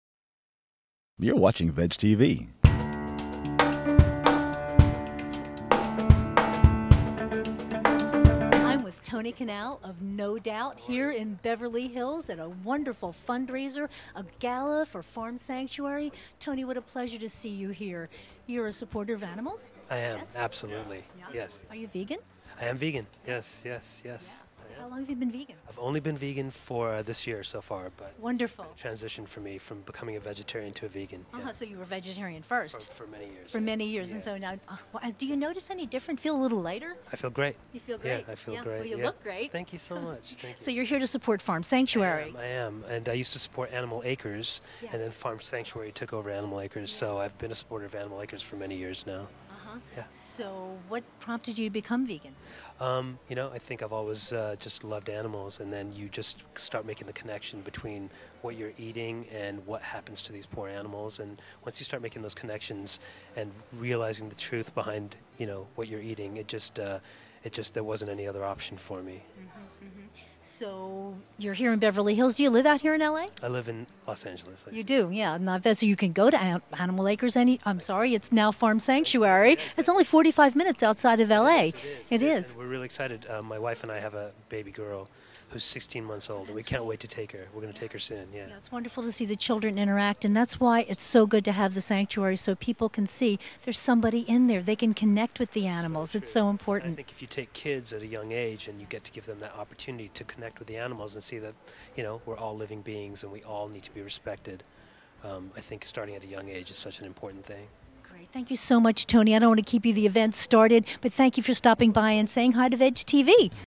Tony Kanal Talks to VegTV at Farm Sanctuary Benefit,June 16,2012
No Doubt bassist Tony Kanal talks to VegTV about animal welfare and being vegan at the Farm Sanctuary Benefit on June 16,2012.